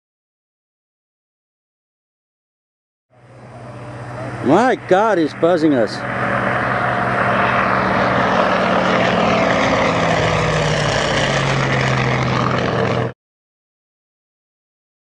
DC-3 buzzing our lodge
A DC-3 is "buzzing" our lodge in SE Alaska. See more fun Alaska lodge clips and stories